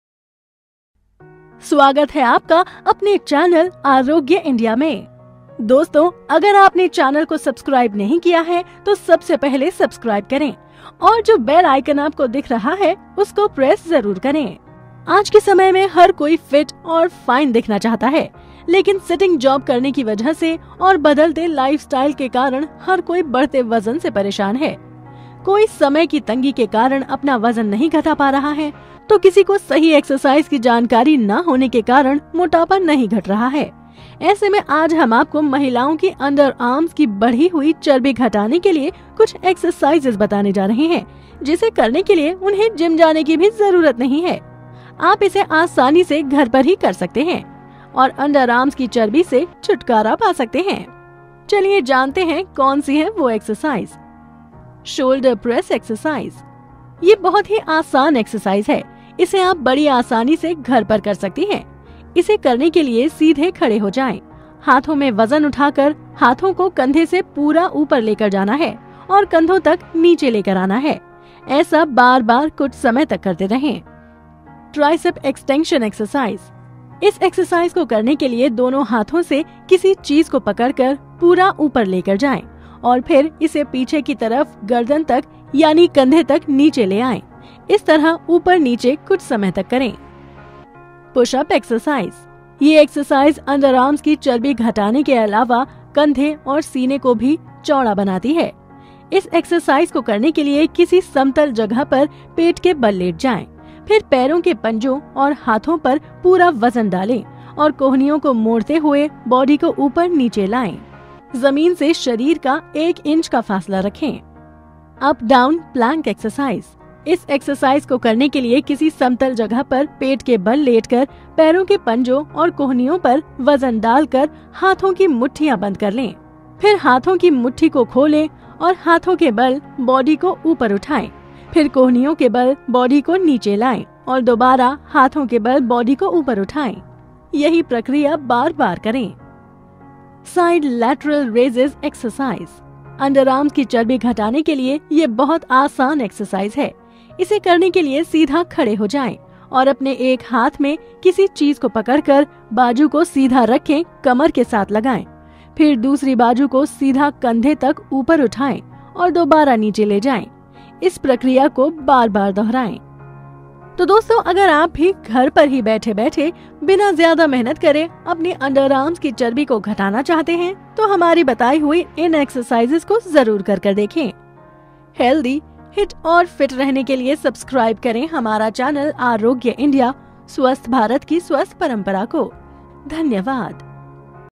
अंडरआर्म्स के पास जिद्दी चर्बी घटाने के लिए करें ये 5 एक्सरसाइज || Arogya India
Copyright : Arogya India